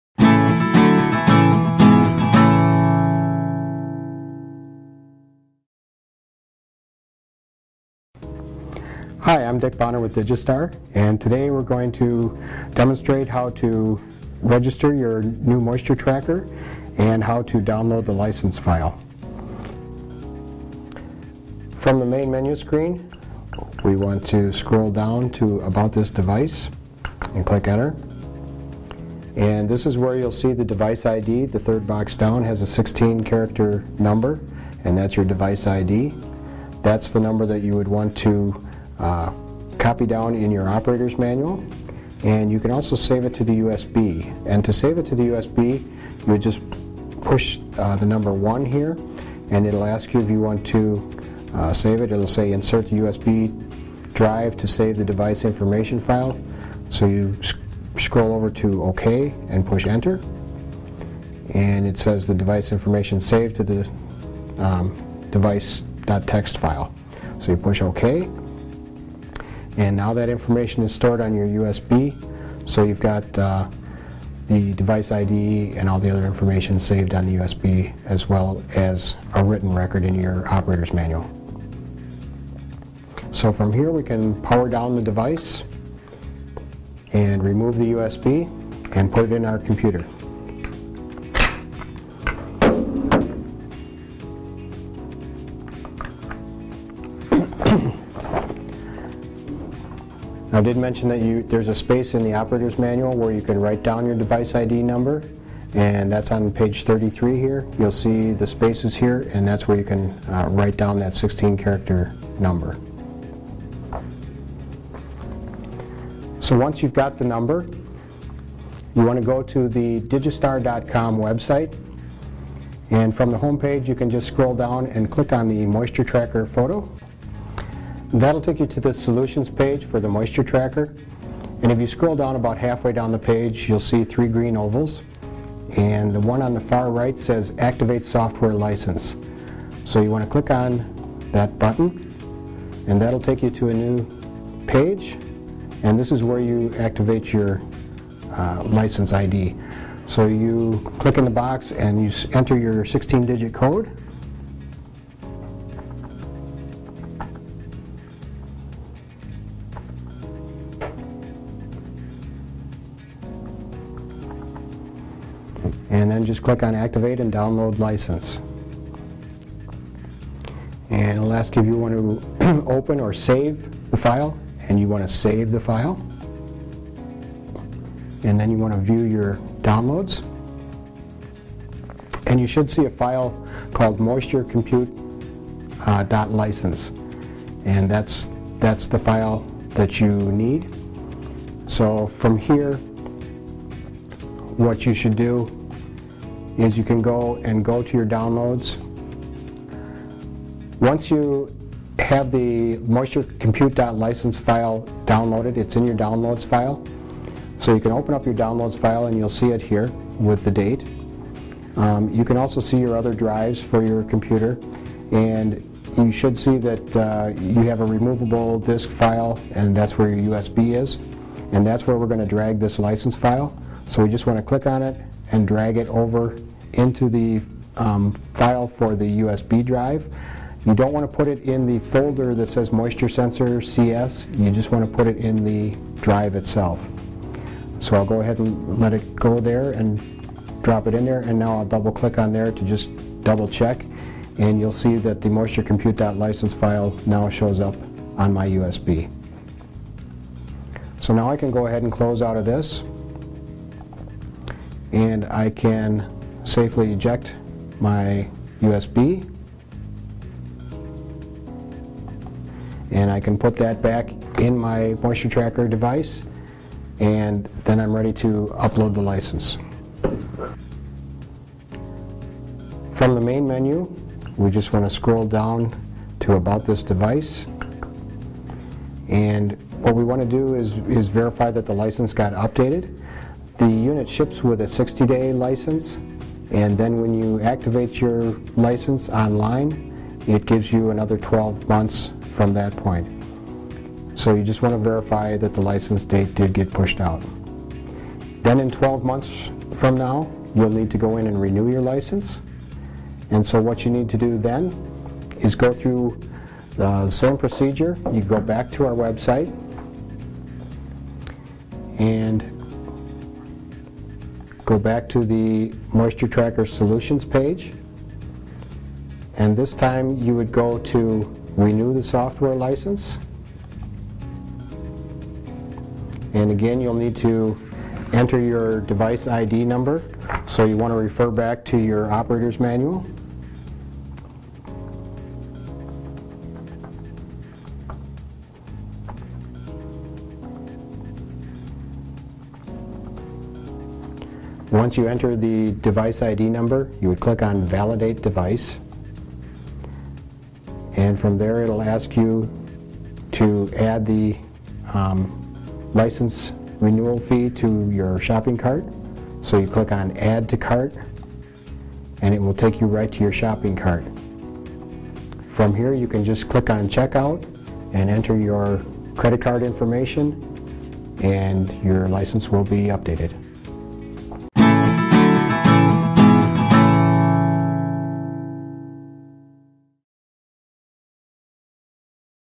Moisture Tracker License Activation
Upon receiving your Moisture Tracker from Digi-Star you must activate your license within 60 days.  This video provides step by step instructions to activate your Moisture Tracker license and how to renew it in 12 months.